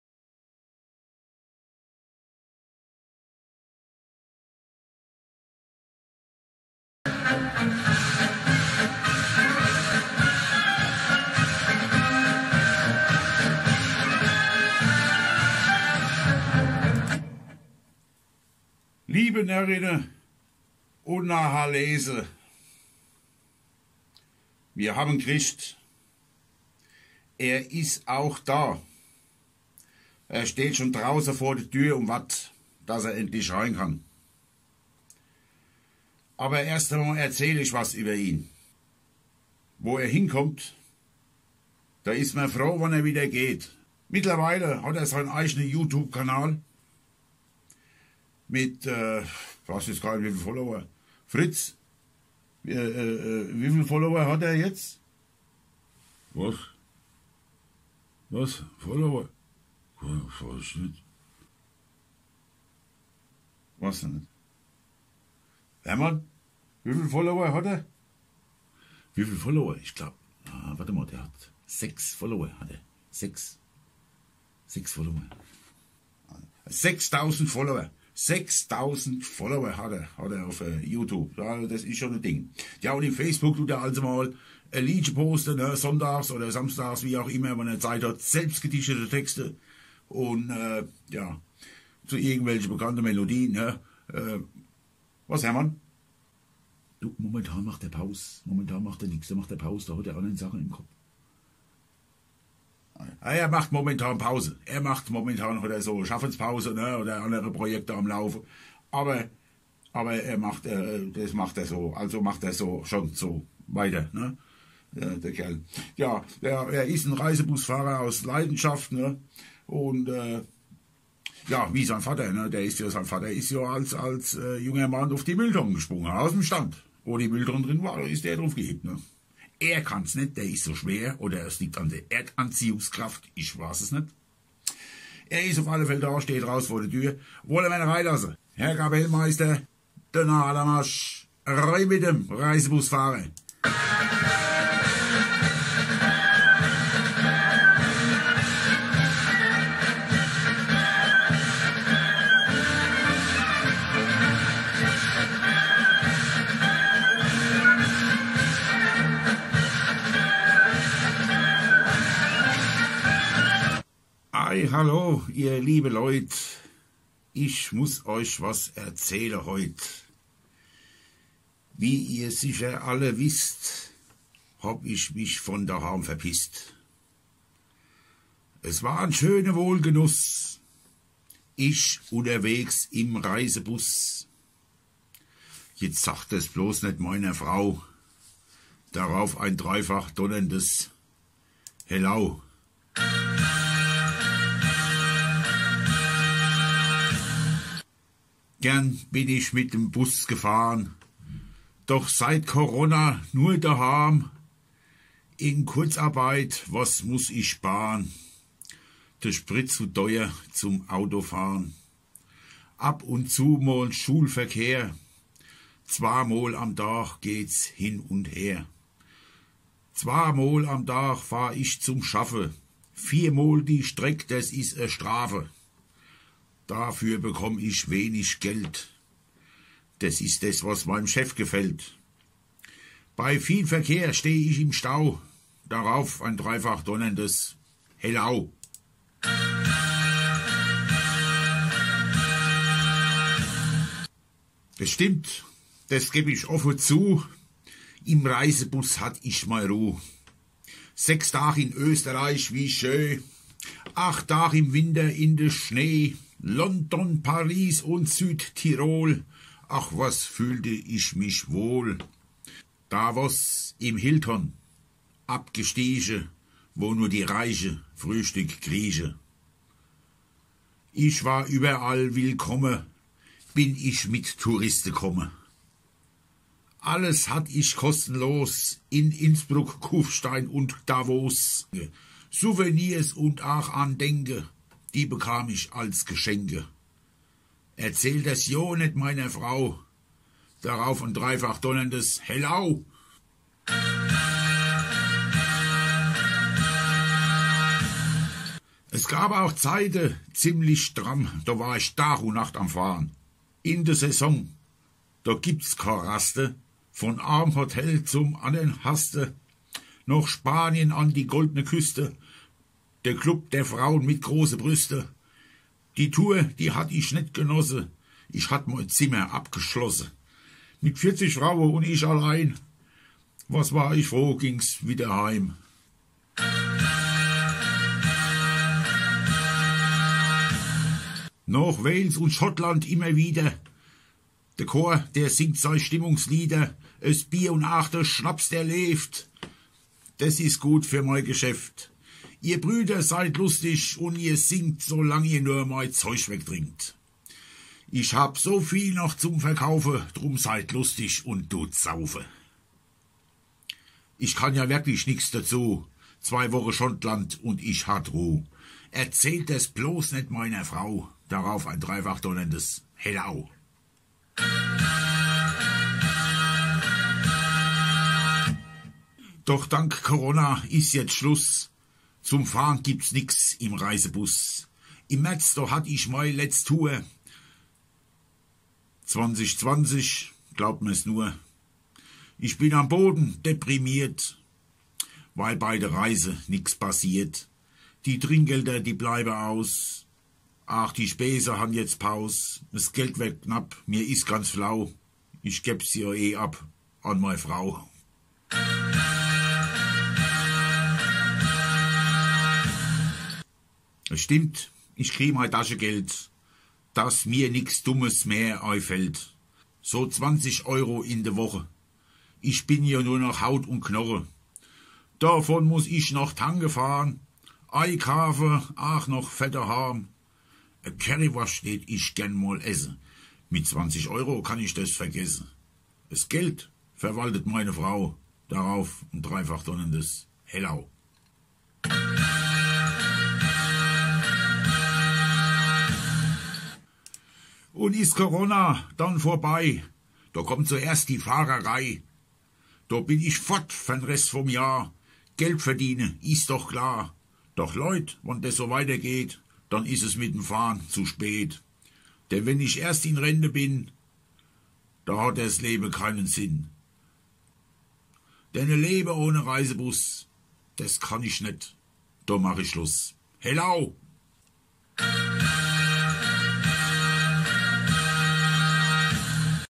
Faschingssitzung mit Roger, Fritz und Hermann
Habe mit Fritz und Hermann eine Faschingssitzung aufgenommen.

Bütteredner ist der Reisebusfahrer